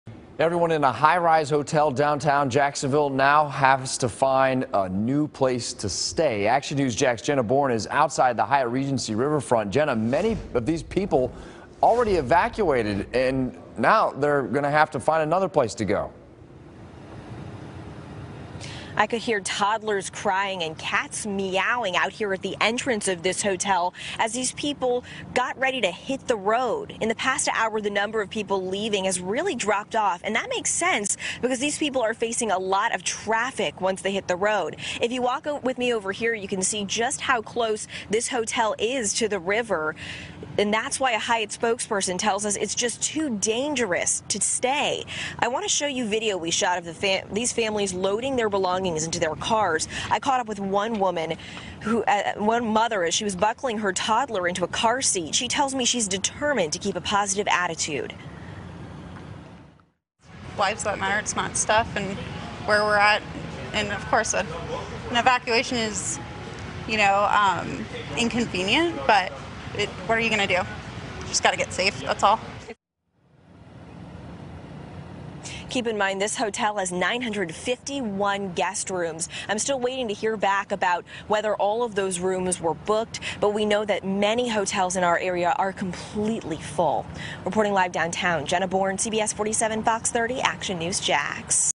Hurricane Irma: Evacuees forced out of downtown Jacksonville hotel
See the latest track on the First Alert Weather Hurricane Center